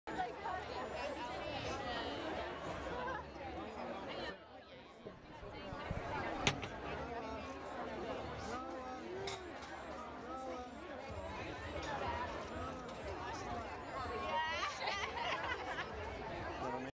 Alessandria Ambrosio at the Third Annual Veuve Clicquot P...
Alessandria Ambrosio at the Third Annual Veuve Clicquot Polo Classic - Los Angeles at Will Rogers State Historic Park on 10/6/12 in Los Angeles, California